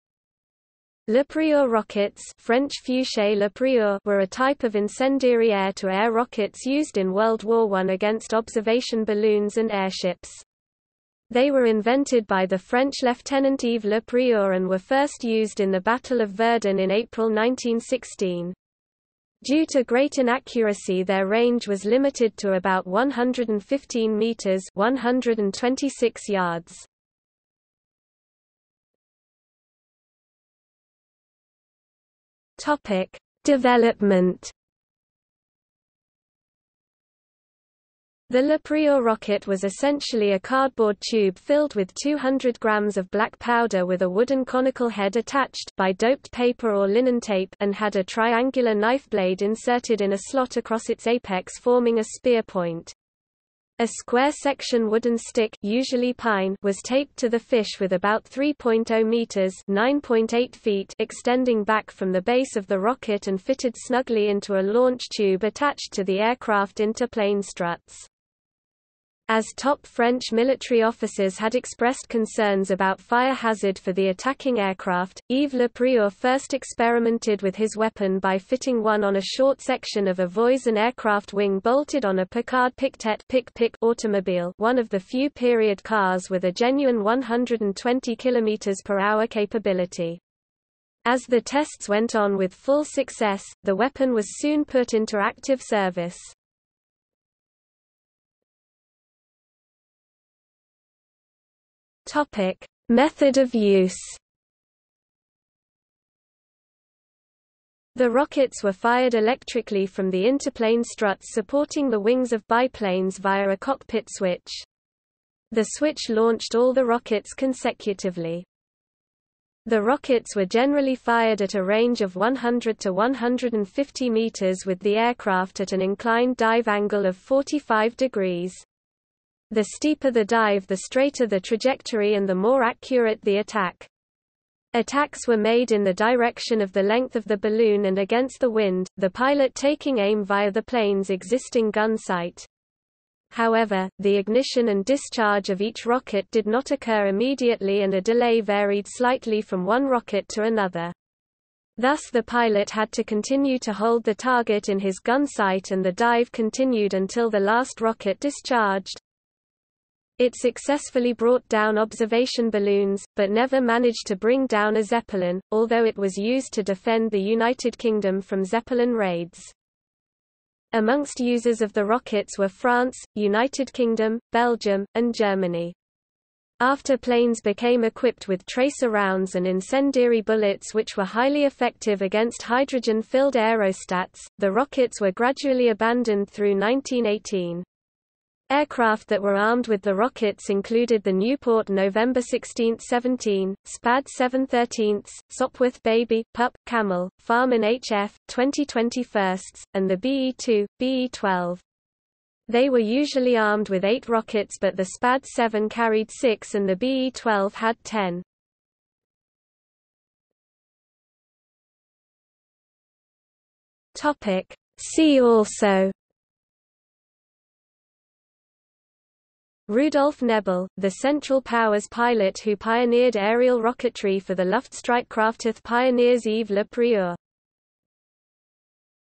Le Prieur rocket | Wikipedia audio article
00:00:28 1 Development
00:01:38 2 Method of use
00:03:30 3 See also

- Socrates

SUMMARY
Le Prieur rockets (French Fusées Le Prieur) were a type of incendiary air-to-air rockets used in World War I against observation balloons and airships. They were invented by the French Lieutenant Yves Le Prieur and were first used in the Battle of Verdun in April 1916. Due to great inaccuracy their range was limited to about 115 metres (126 yards).